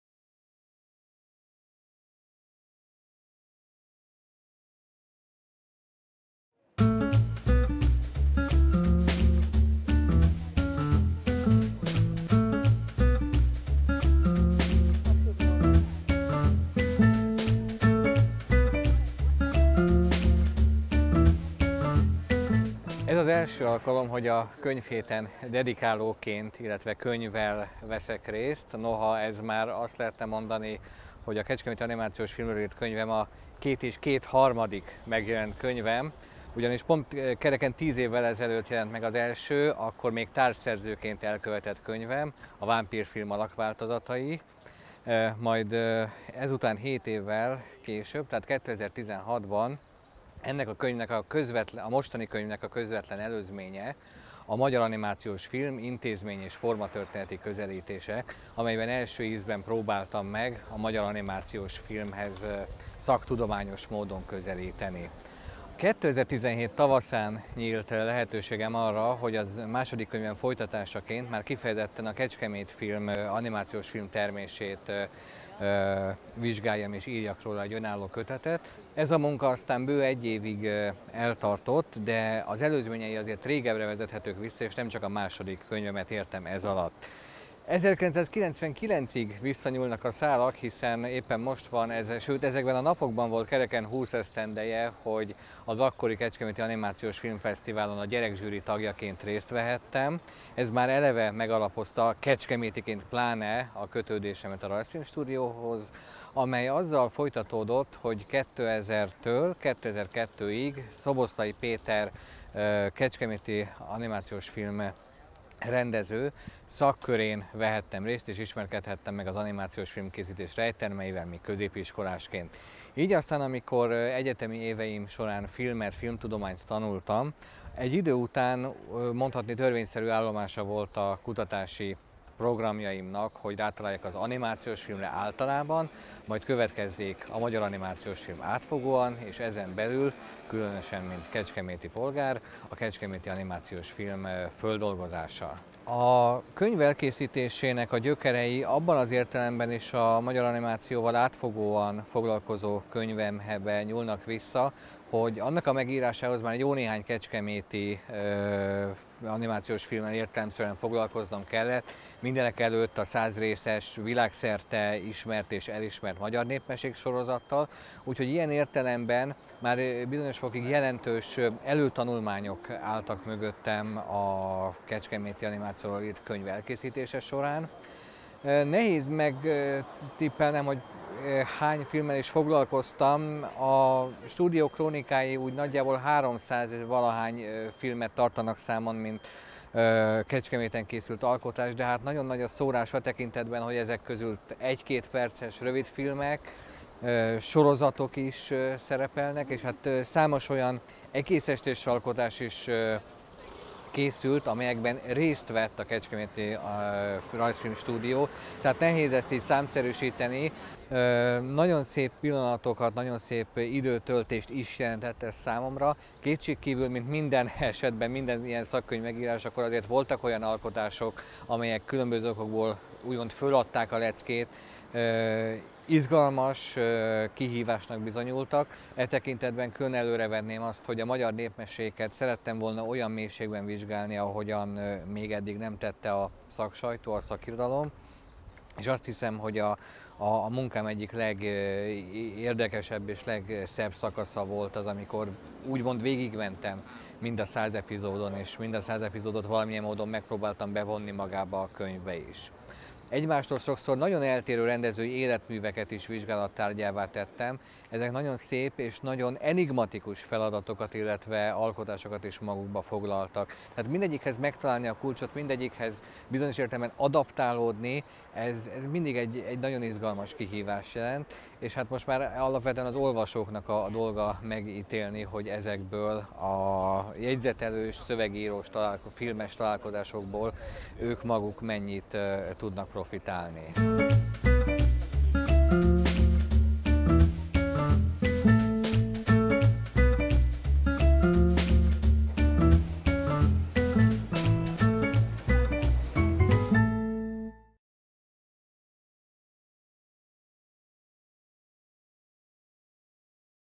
Könyvheti beszélgetések – Varga Zoltán dedikál
A 90. Ünnepi Könyvhéten Varga Zoltán dedikálta a Kecskeméti animációs film című könyvét.